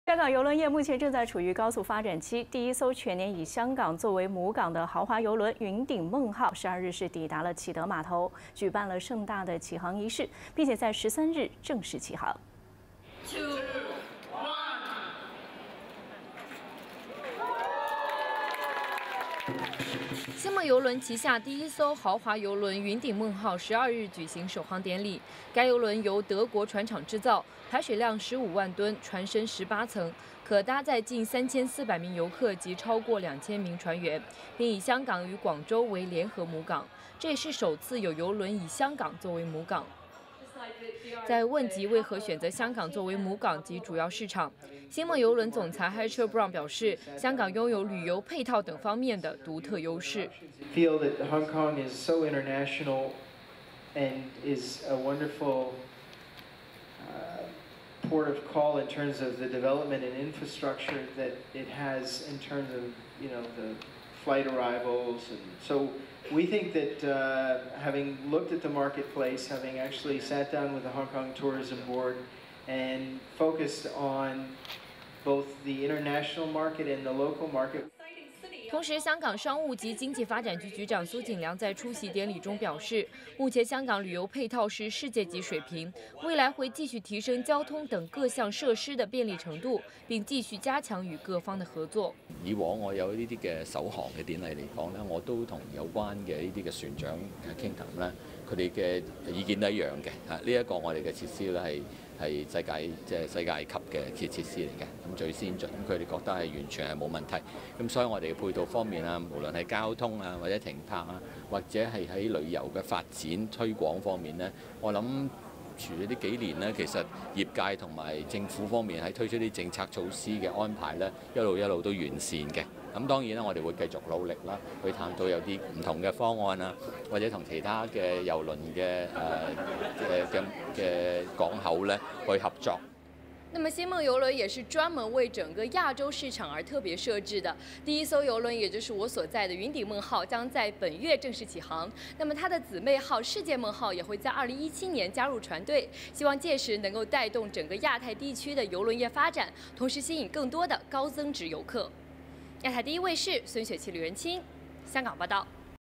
全年以香港為母港 “雲頂夢號”香江啟航
星夢郵輪旗下第一艘浩華郵輪雲頂夢號12日舉行首航典禮，該郵輪由德國船廠製造，排水量15萬噸，船身18層，可搭載近3400名遊客及超過2000名船員，並以香港及廣州為聯合母港，也是首次有郵輪以香港為母港。